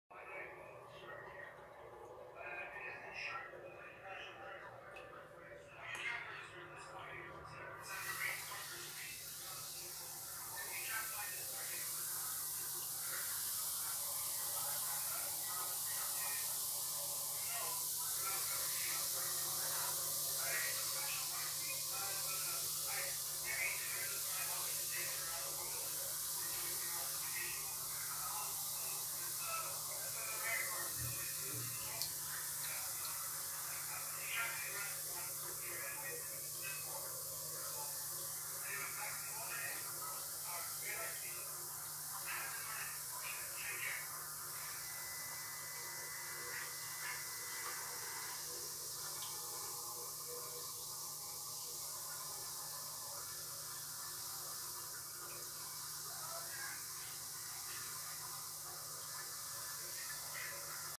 Bear Necessities Bath Bomb Blaster
Cruelty free and 100% handmade bath bombs, watch it fizz in this video and get yours